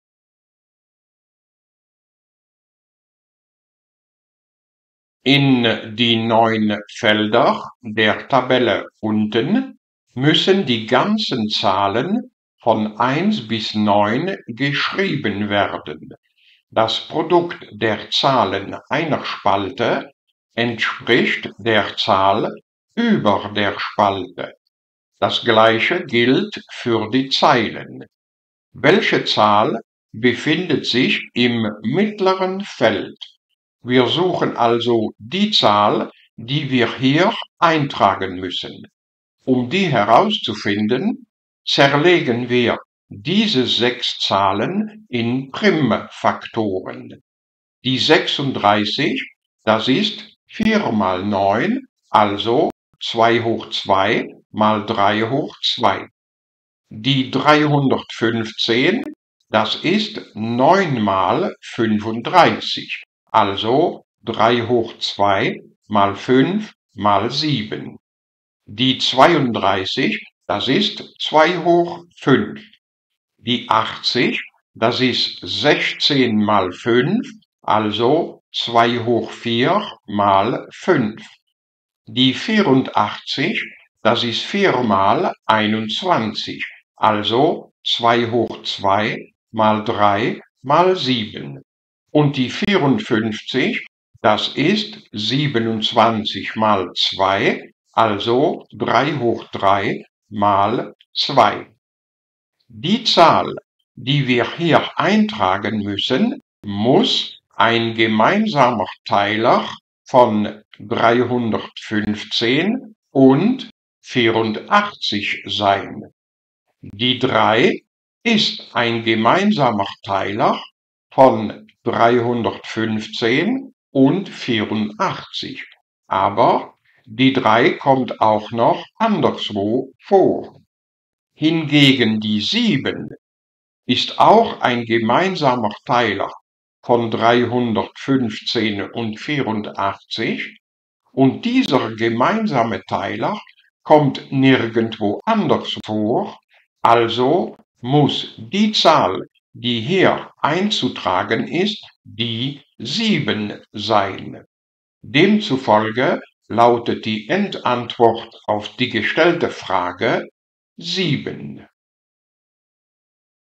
Mathematik-Olympiade 2024 Maxi 065
Wir lösen eine Aufgabe aus der belgischen Maxi-Mathematik-Olympiade 2024